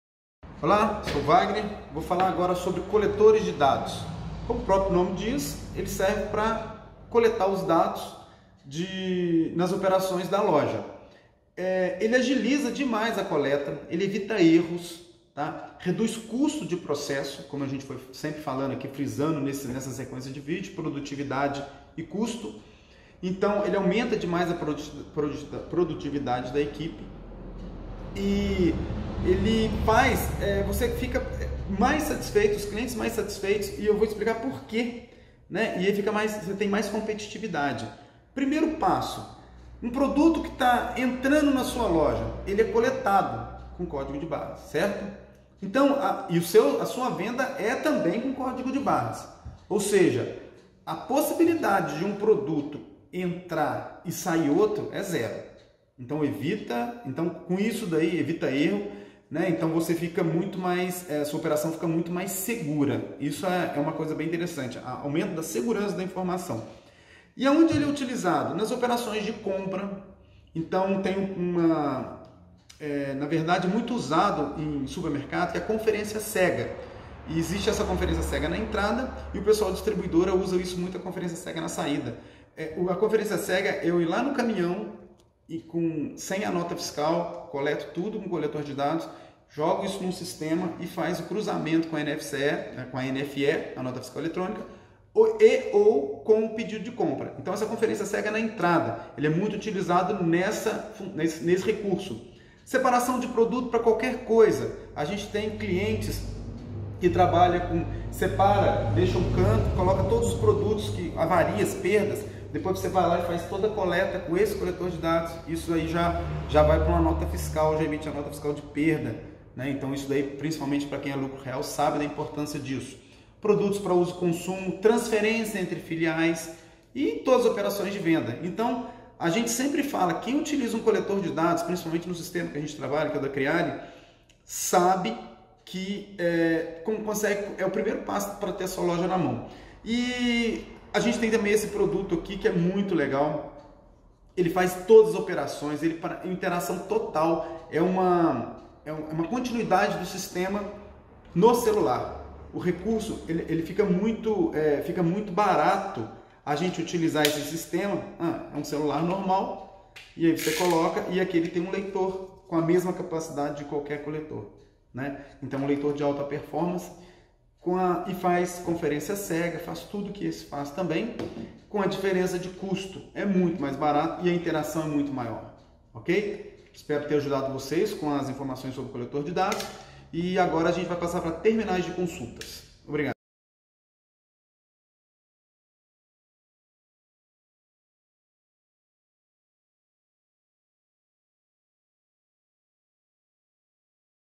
Coletor de dados
Agilize sua operação de forma simples e prática, levando mais economia e segurança para seus negócios com coletor de dados.
Estes equipamentos foram projetados para as mais diversas operações: logísticas, fabricação, inventários, mercado de varejo e outras.